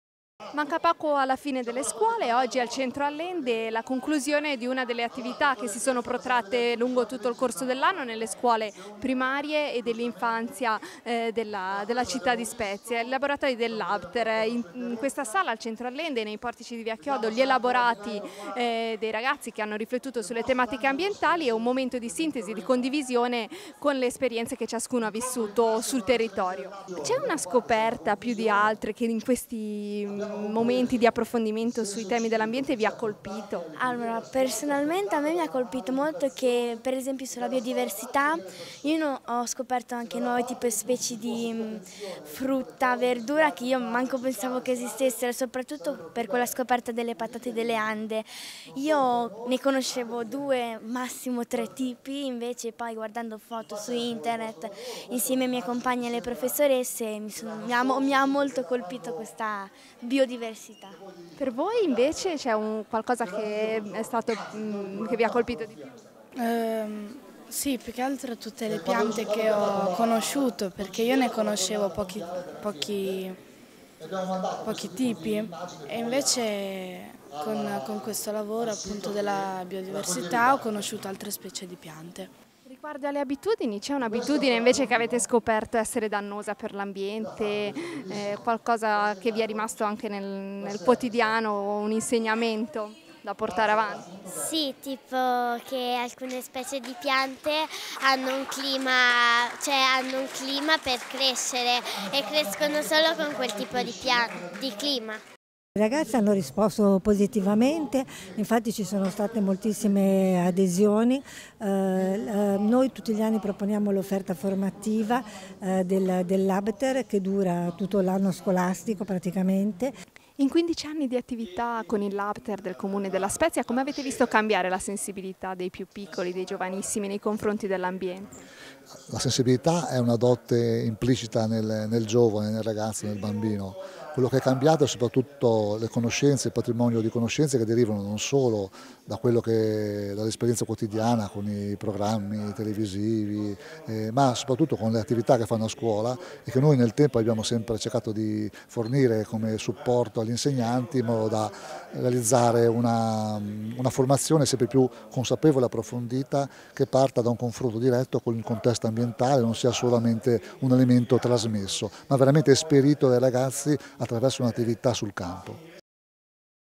Labter, conclusi i corsi 03-06-2016
Tabelloni, video, prodotti multimediali, presentazioni interattive online; sono le più varie le forme di comunicazione adottate dalle scuole, attraverso le quali i ragazzi si sono confrontati con esperti dei vari ambiti tematici affrontati.
Stamani al centro allende sono stati presentati gli elaborati della quattordicesima edizione del Progetto Ambiente,  momento di costituzione del Laboratorio di Educazione Ambientale del Comune della spezia